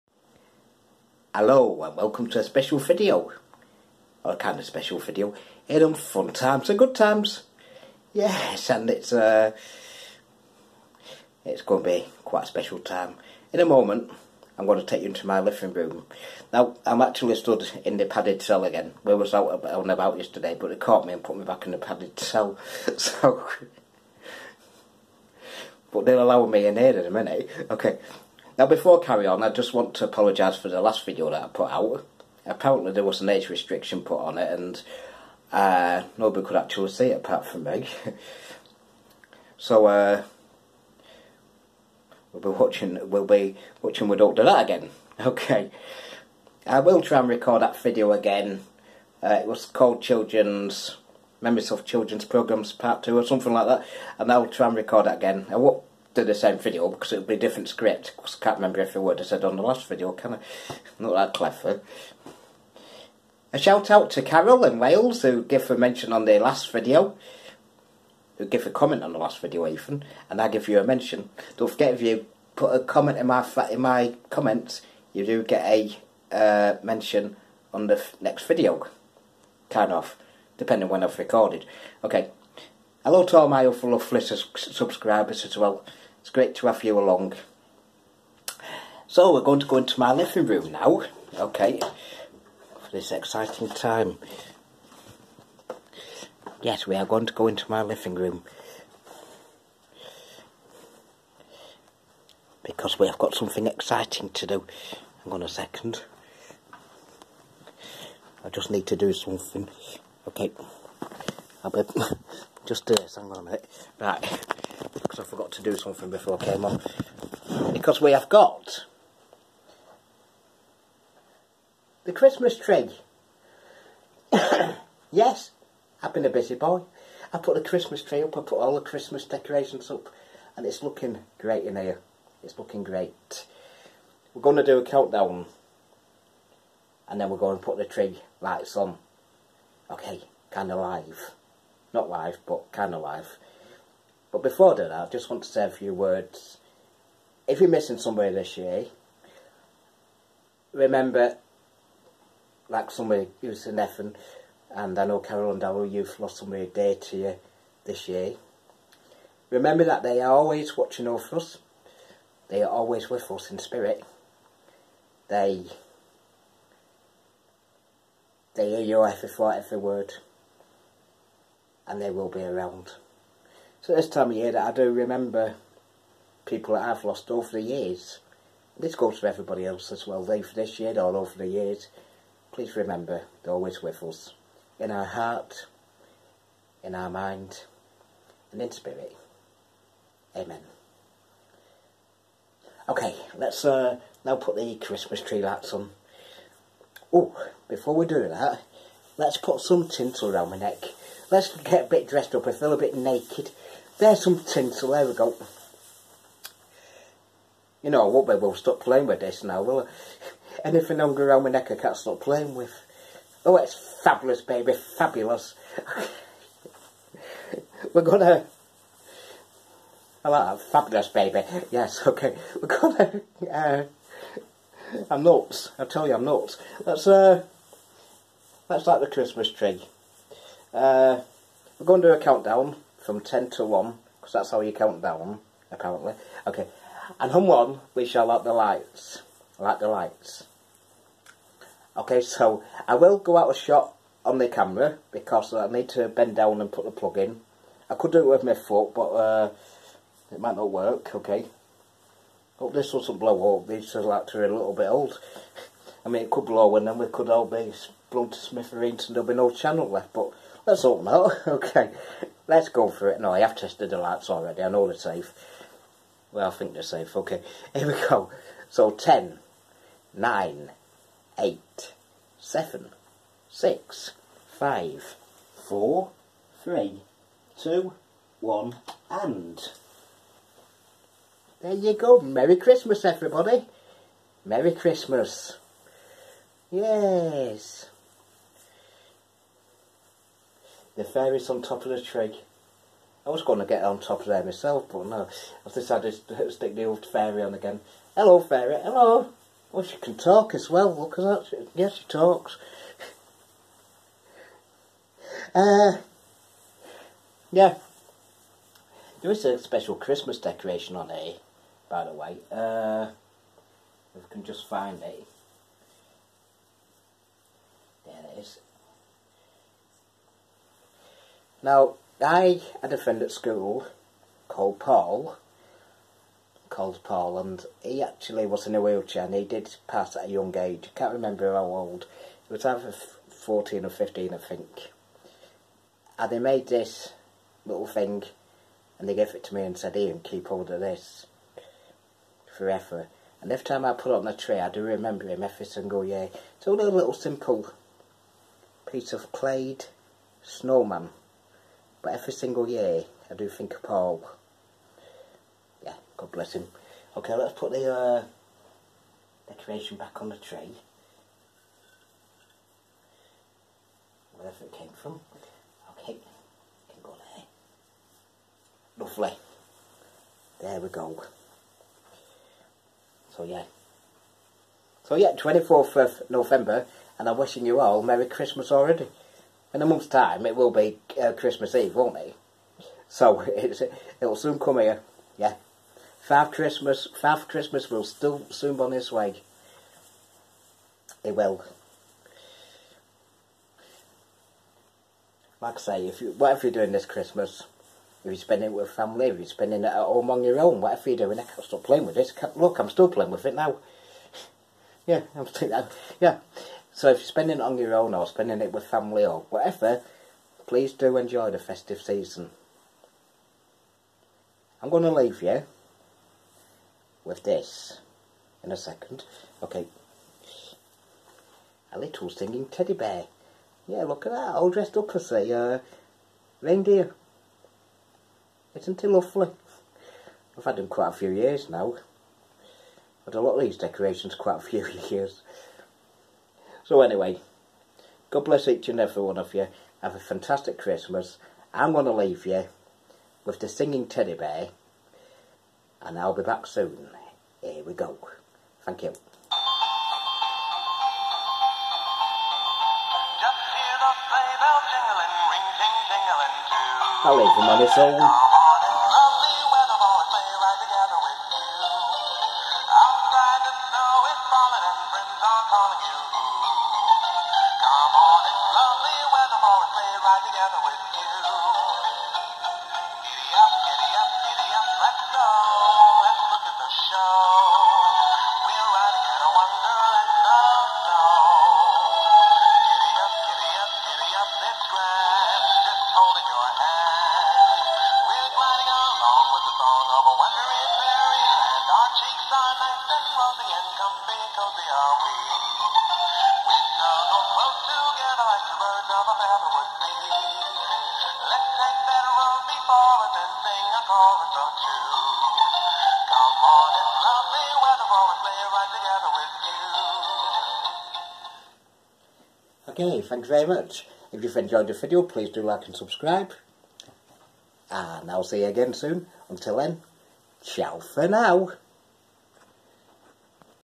It's Christmas, well nearly lol
Join me as I start my Christmas celebrations here in Lancashire
Hope everyone's well X

Ian